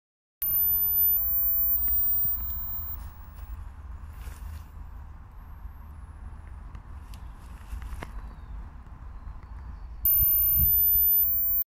Dormouse in my garden
I spotted this dormouse scurrying around in my garden.
(old video from 2019)